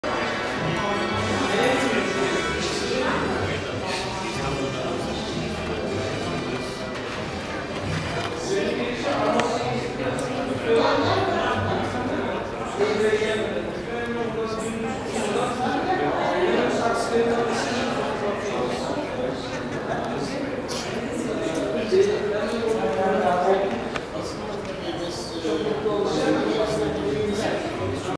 Prof. Dr. Nadir Devlet TRT Tatar'da
Prof. Dr. Nadir Devlet 7 Kasım 2015'te Ankara Kazan Kültür ve Yardımlaşma Derneği'nin 50. kuruluş yıldönümü balosuna katıldı. Mövenpick Otelde düzenlenen tören öncesinde, Prof. Devlet TRT Tatar'a röportaj verdi.